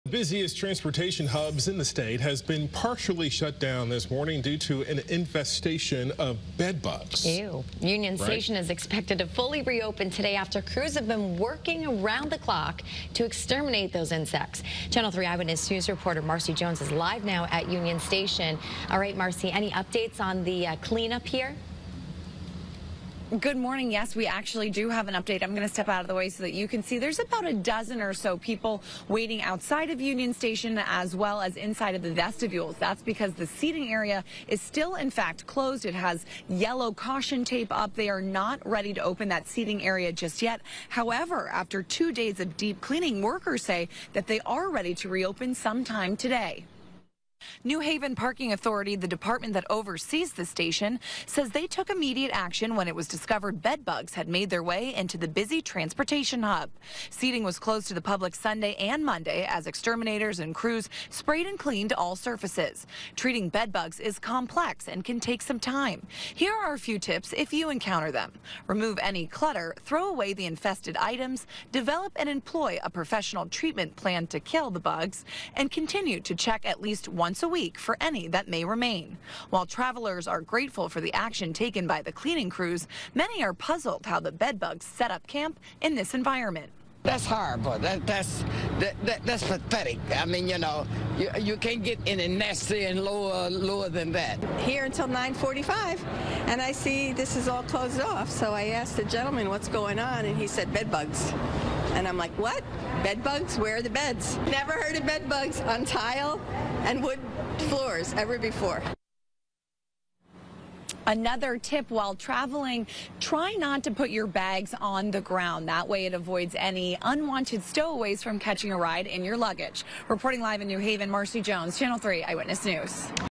Reported bed bugs at Union Station shuts down seating area
New Haven’s Union Station has no seating for travelers Monday evening after reports of bed bugs.